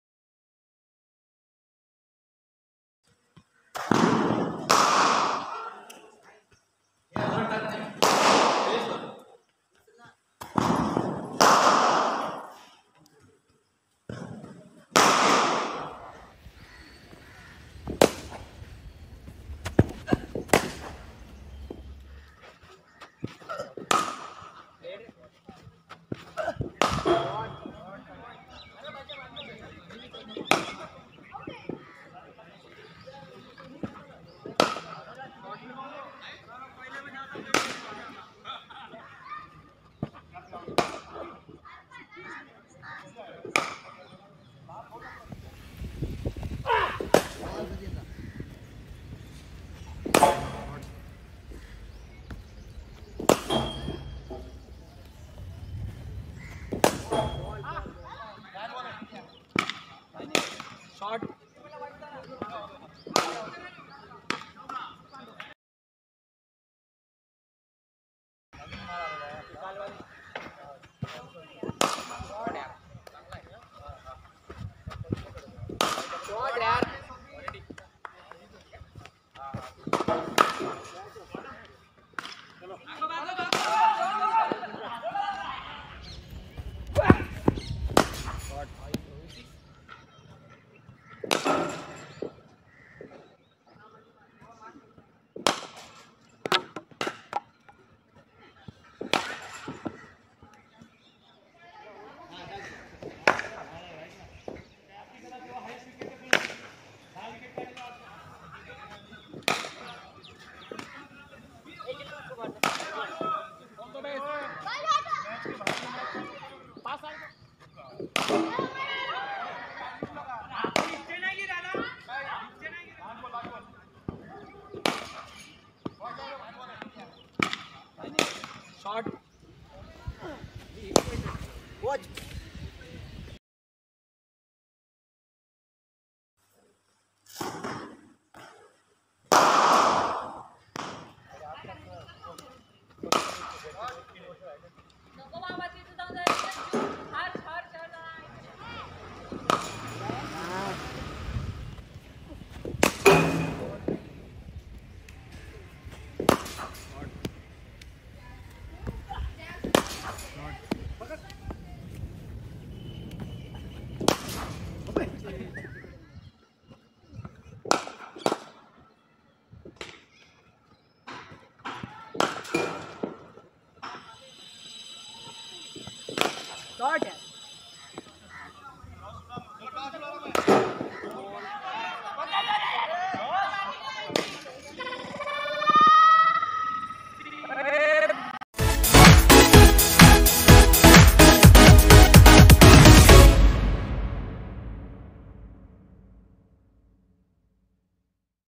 Best cricket shots | my favourite cricket shots | top 3 cricket shots
In this video you will see my top 3 favourite cricket Shots.
I love to play cover drive, pick up Shot and step out lofted shots.
cricket practice batting.

Host - IG - @_swapnil_ingale

Synthetic Balls
Sidearm thrower
My cricket pads
My Cricket gloves
My cricket helmet
Cricket Spike shoes
I use Addidas